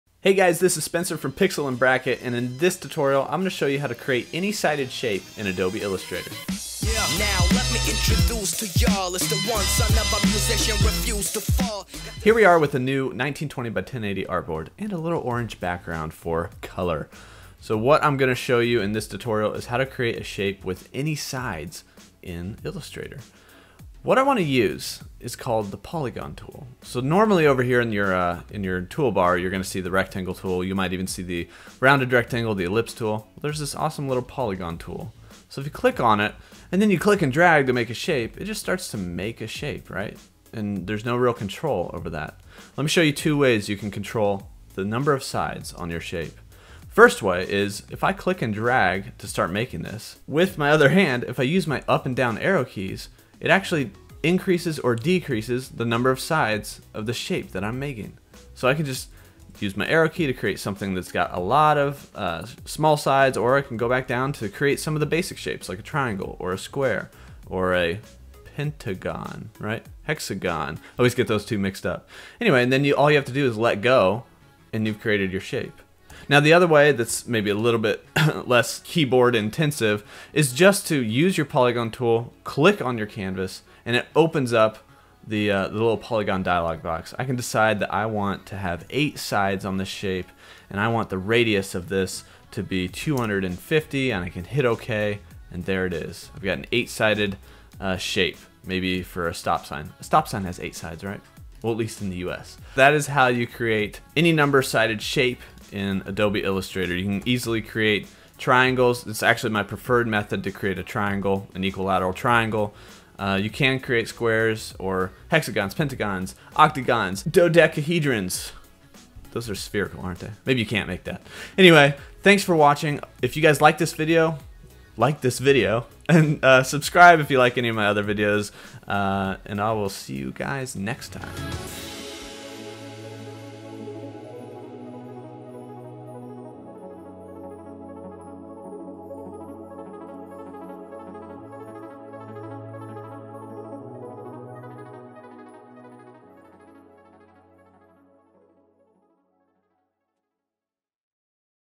How to Make Shapes with the Polygon Tool | Illustrator Tutorial
In this Illustrator tutorial learn how to make shapes with the polygon tool. This polygon tool tutorial shows you how simple it can be to make shapes with any number of sides!